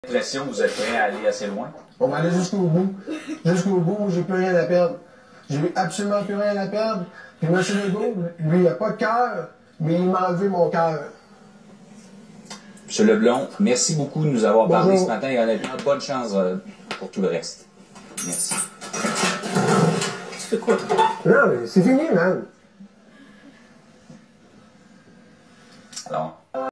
Un chauffeur de taxi s’ouvre les veines à LCN :(
Désespéré, Un chauffeur de taxi s’ouvre les veines à LCN :( ce 29 mars 2019.